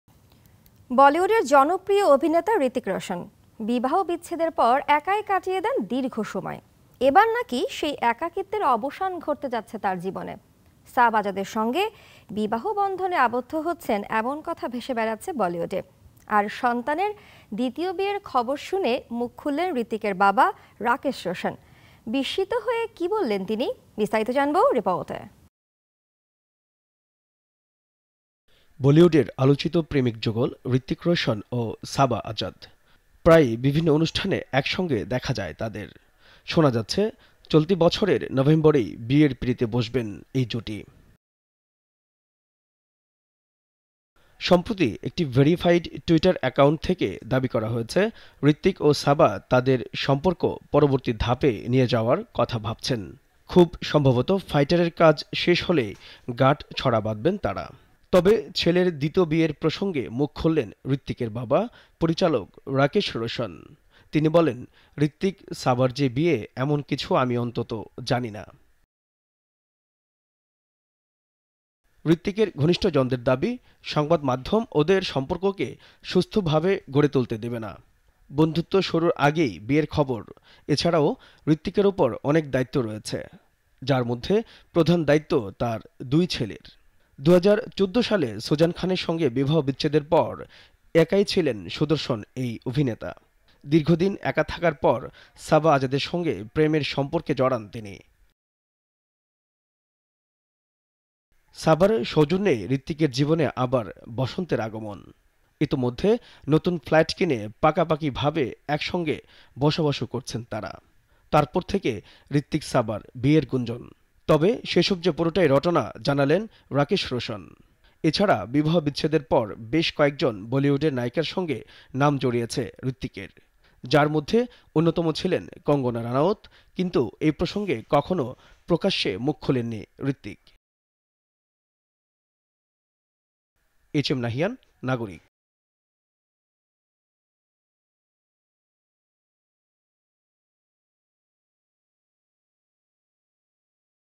ছেলের দ্বিতীয় বিয়ের খবর শুনে যা বললেন হৃতিকের বাবা | Hrithik Roshan | Relationship | Nagorik TV
ছেলের দ্বিতীয় বিয়ের খবর শুনে যা বললেন হৃতিকের বাবা...

বলিউডের জনপ্রিয় অভিনেতা হৃতিক রোশন। বিবাহ বিচ্ছেদের পর একাই কাটিয়ে দেন দীর্ঘ সময়। এবার নাকি সেই একাকিত্বের অবসান ঘটতে যাচ্ছে তার জীবনে। সাবা আজাদের সঙ্গে বিবাহ বন্ধনে আবদ্ধ হচ্ছেন এমন কথা ভেসে বেড়াচ্ছে বলিউডে। আর সন্তানের দ্বিতীয় বিয়ের খবর শুনে মুখ খুললেন হৃতিকের বাবা রাকেশ রোশন। বিস্মিত হয়ে কী বললেন তিনি?